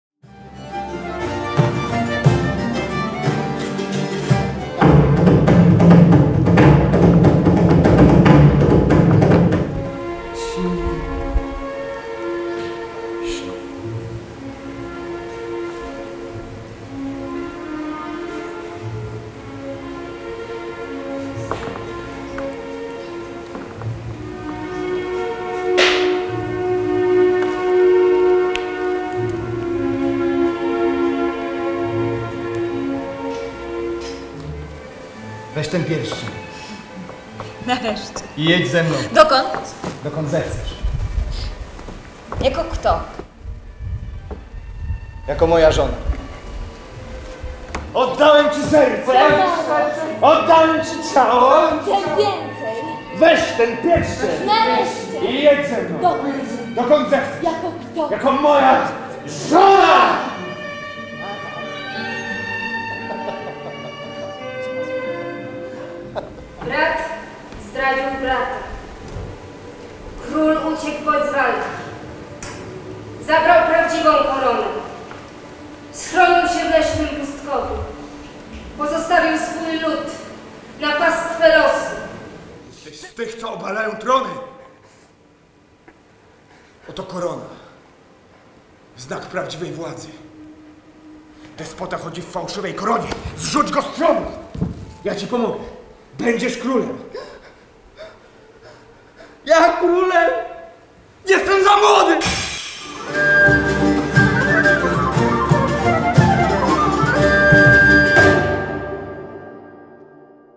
🎭 Hijas del aire. Sueño de Balladyna 🎭Almagro42
“Hijas del aire. Sueño de Balladyna”, adaptación a cargo de Marta Eloy-Cichocka (Polonia) y con la dramaturgia de José Gabriel López Antuñano (España) y Anna Galas-Kosil (Polonia), combina dos dramas canónicos en nuestras culturas confrontando en el escenario dos personajes femeninos increíblemente fuertes
La 42 edición del Festival Internacional de Teatro Clásico de Almagro.